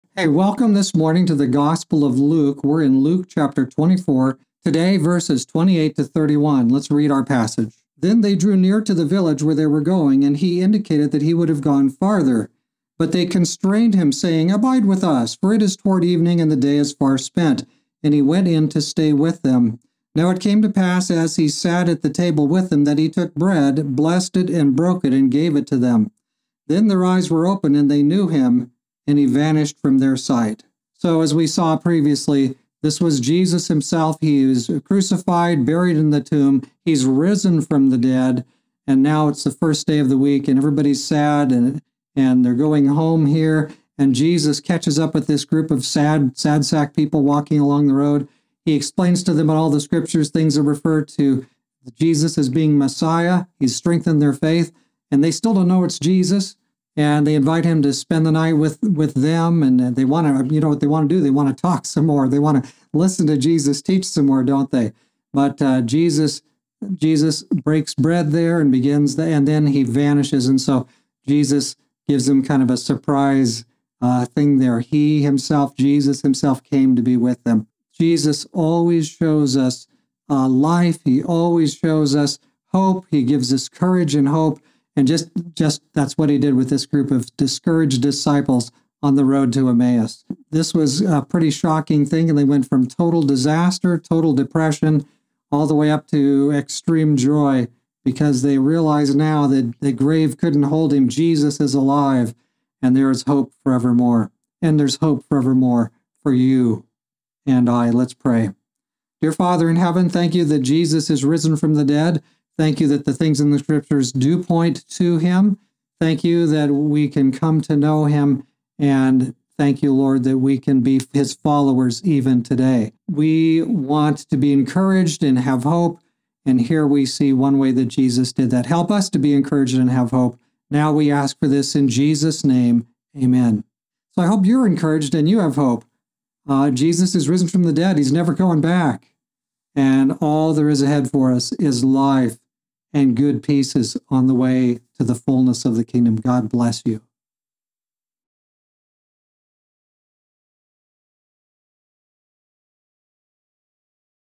Luke 24:28-31 Jesus Revealed
Jesus reveals that it is Himself who has come to encourage His disciples and then vanishes from their sight.